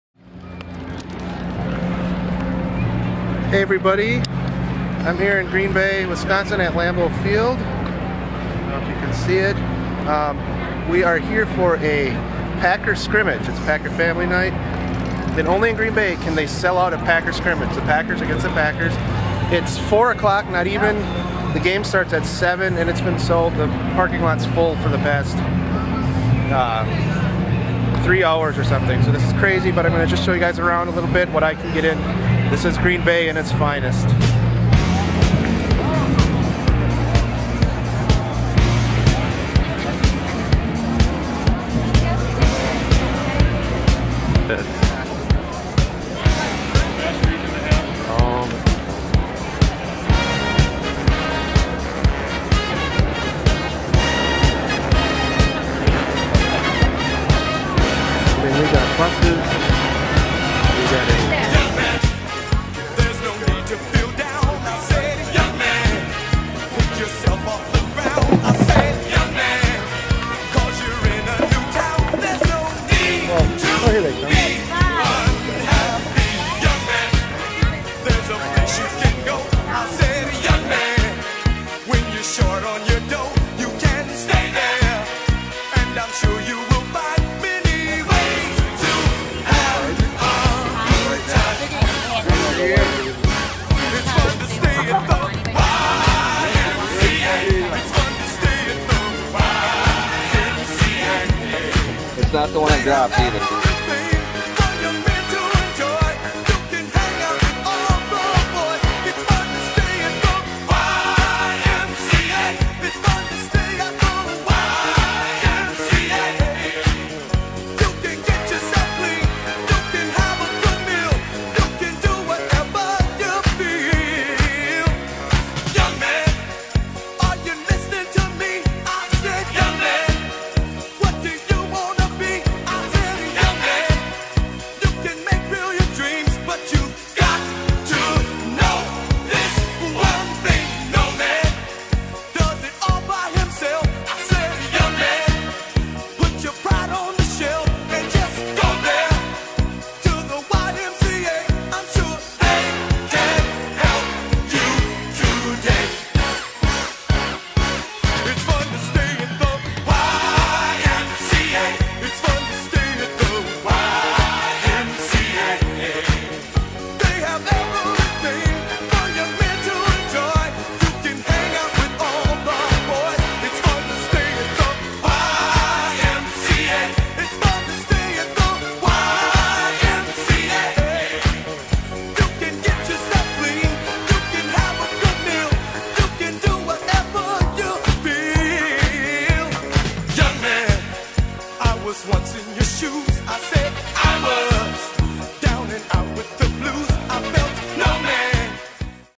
08.04.07: A Taste of Green Bay
Here's some footage I shot from the Packers family night.